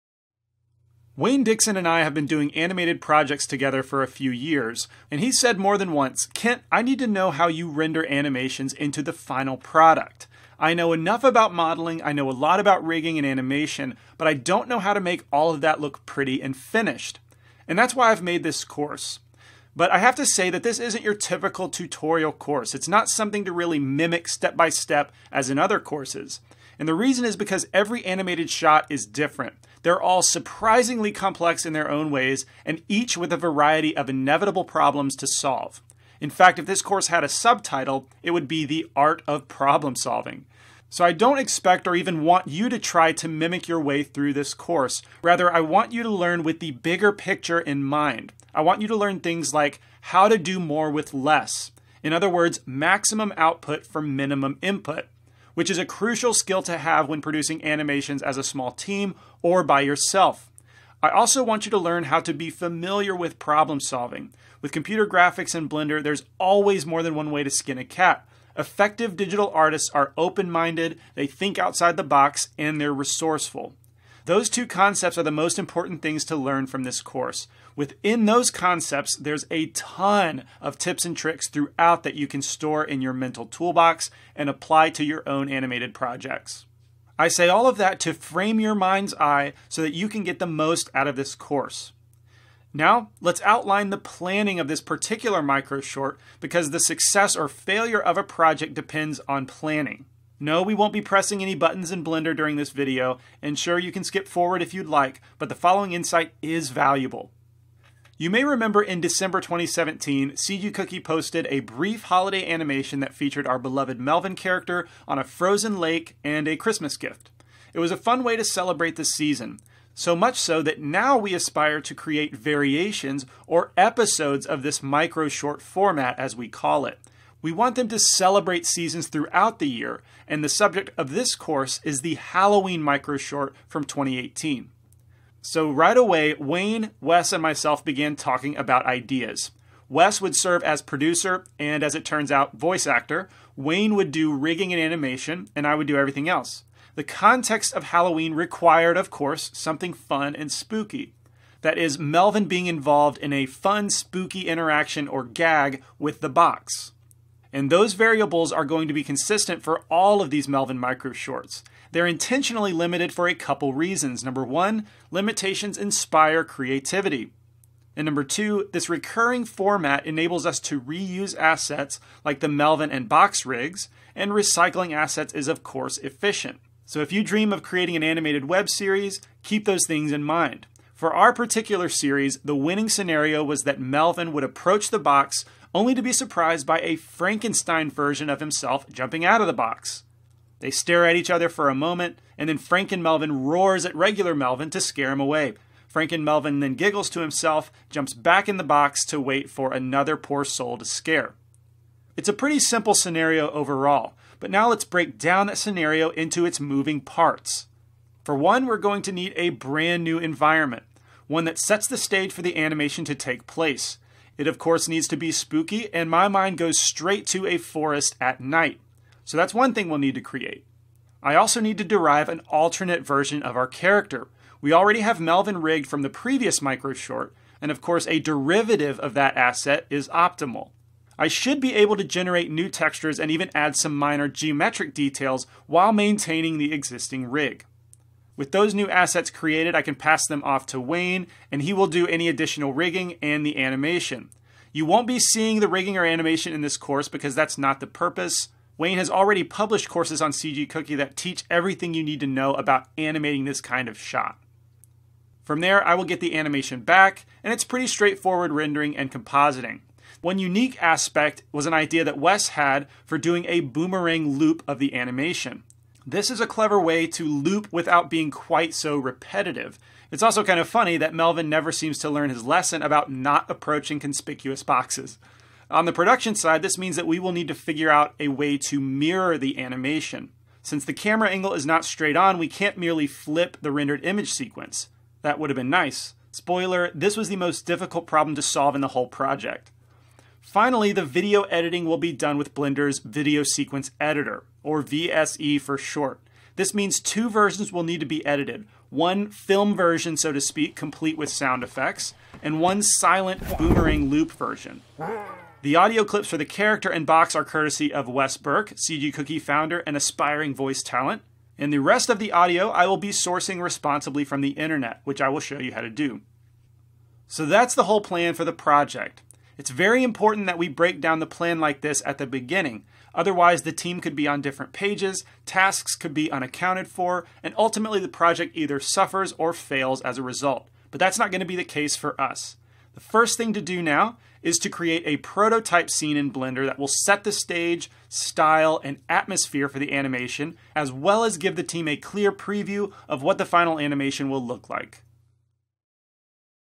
Planning an Animated Short Film - Rendering & Finishing a Blender Animation Pt. 1
Wayne Dixon and I have been doing animated projects together for a few years. And he’s said more than once, “Kent I need to know how you render animations into the final product. I know enough about modeling, I know a lot about rigging and animation but I don’t know how to make all of that look pretty and finished.”

It’s a lot of work to finish an animation and look nice. But the process isn’t over when the animation is complete. It’s also a lot of work to get the animation shaded, lit, and rendered so it’s presentable and beautiful. That’s what this course is all about: Not how to animate but how to make your animations beautifully rendered.